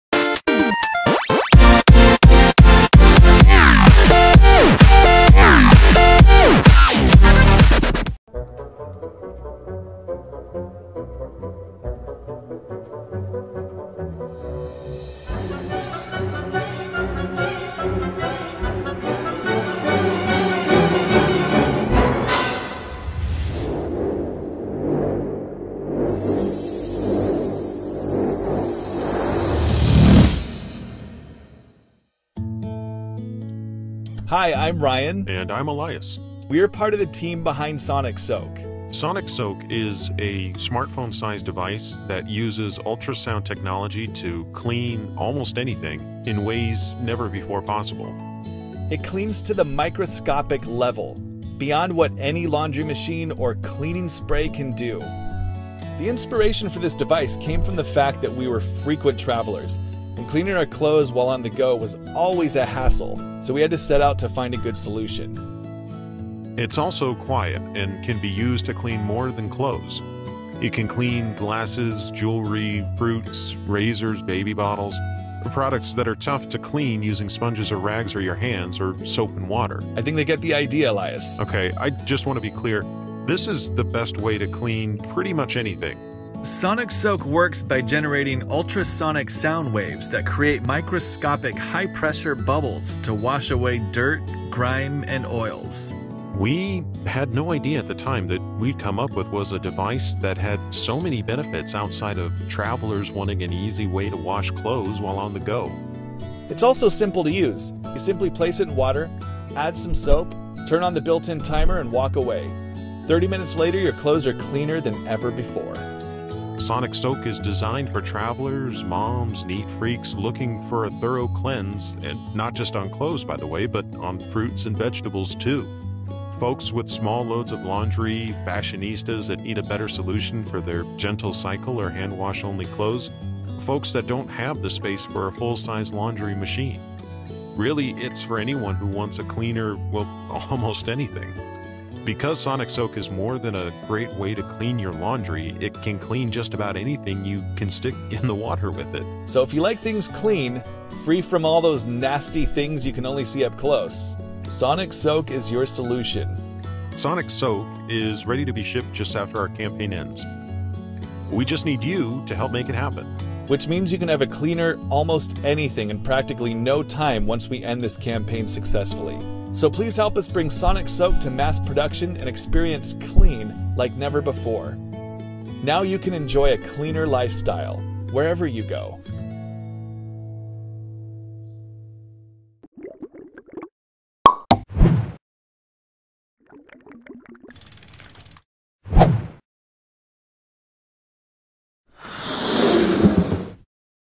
Sonic Soak: The Ultimate Ultrasonic Cleaning Tool
What is Sonic Soak?
Sonic Soak is a device that aims to revolutionize cleanliness and hygiene in our daily lives by using ultrasonic cleaning technology. Sonic Soak's modulated ultrasonic waves travel through water to clean at a microscopic level.

For more Tech, Tips & Tricks